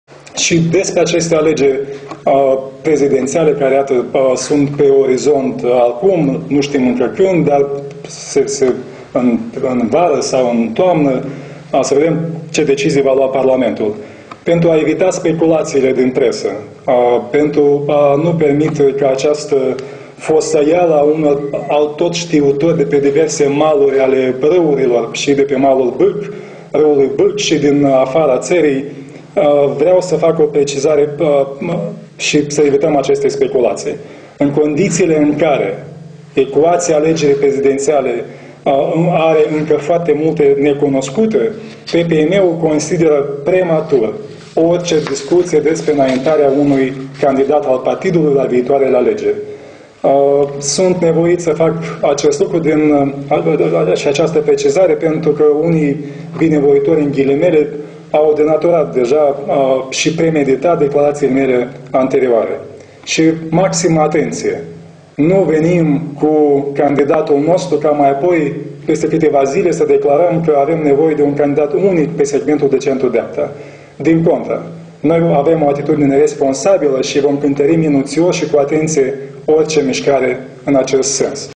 Conferință de presă dedicată aniversării unui an din momentul lansării inițiativ...
Iurie Leancă: PPEM nu are &#238;ncă un candidat la prezidențiale.
Titlul evenimentului: Conferință de presă dedicată aniversării unui an din momentul lansării inițiativei pentru fondarea Partidului Popular European din Moldova